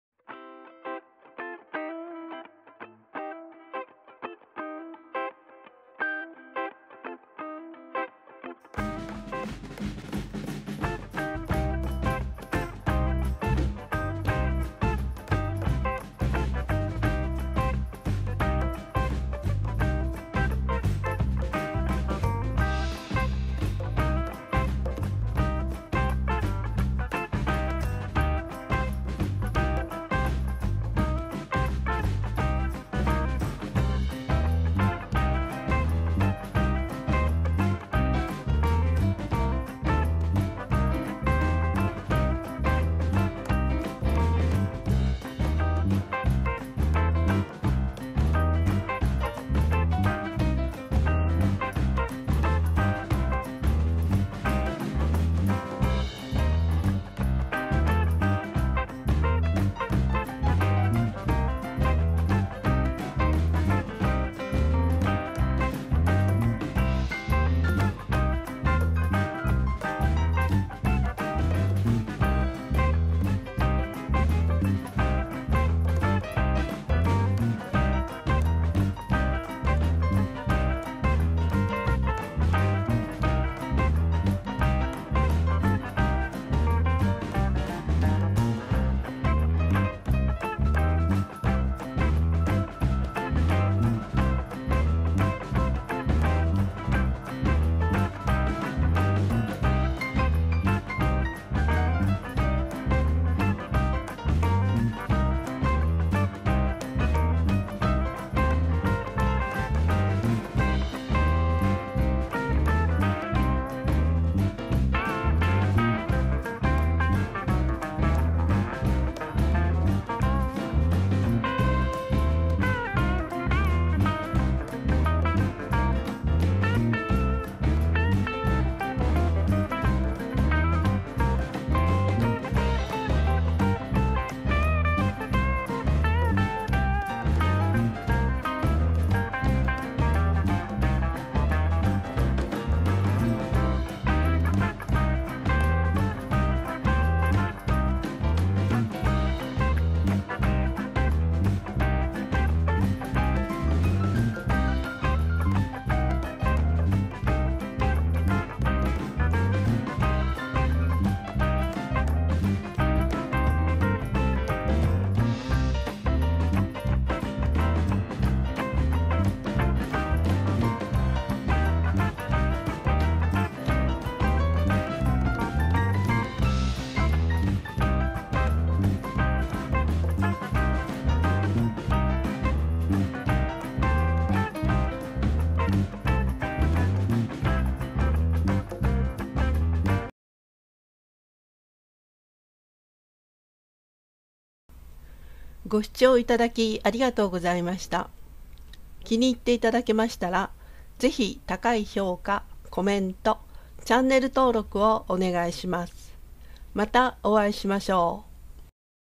面白くておかしくて頑張ってるかわいい動物たち-2
犬やネコの動画を集めました。
かわいいのやら、情けないのやらが17匹います。
笑いながら見てください。